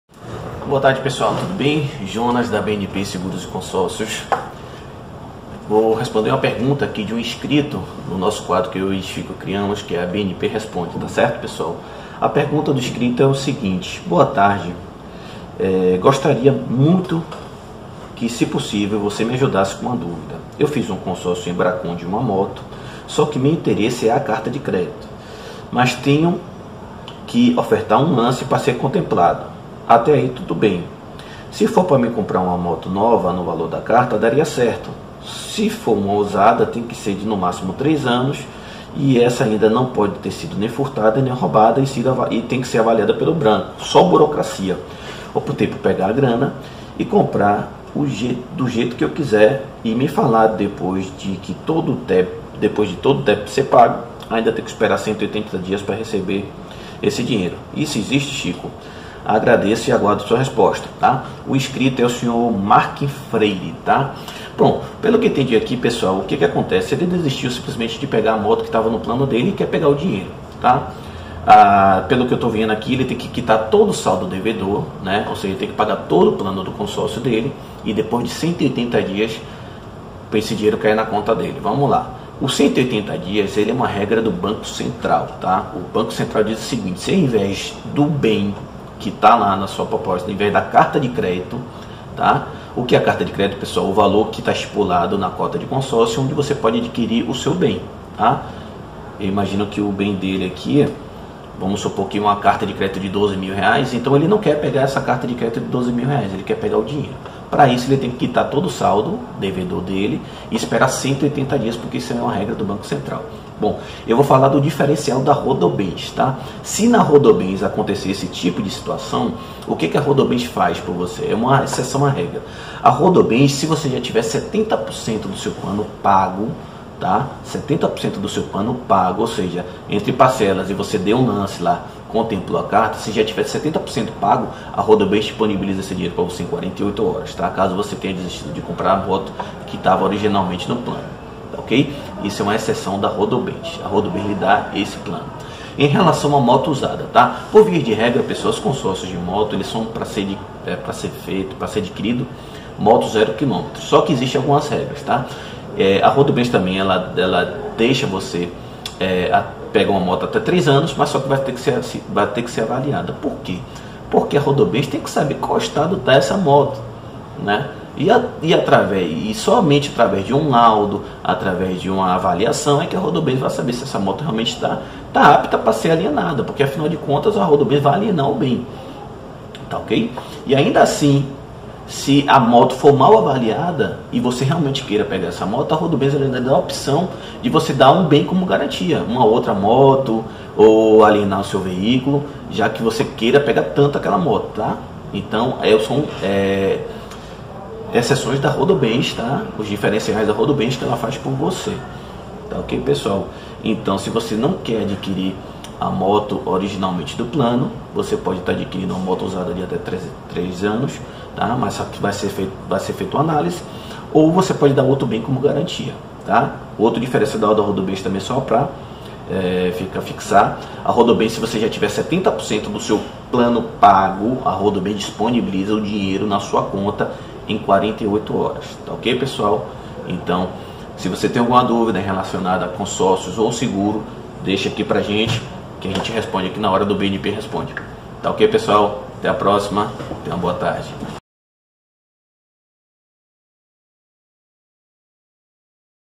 BNP Responde: O que é CARTA DE CRÉDITO? Posso comprar uma moto usada com a carta de crédito?
Mega Motos - Concessionária Dafra, Shineray e Bull